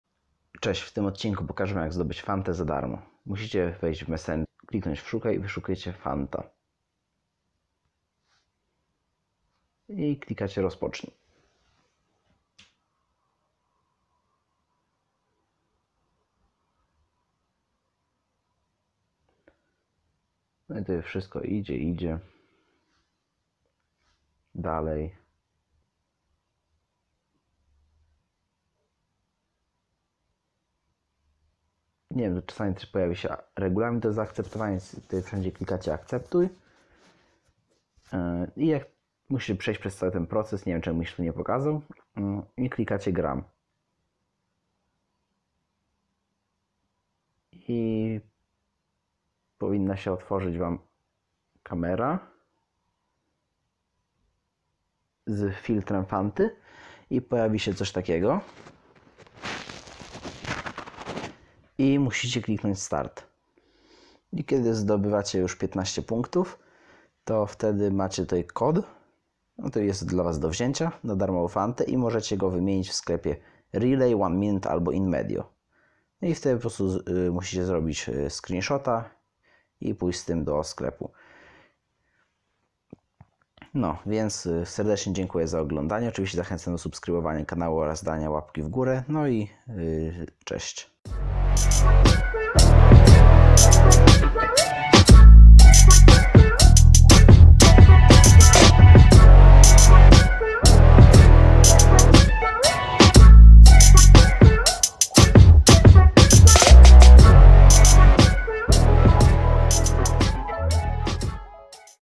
Jak zdobyć Fantę ZA DARMO
Hej,  dzisiaj zobaczycie, jak zdobyć darmową fantę. Tego sposobu możesz używać bez ograniczeń!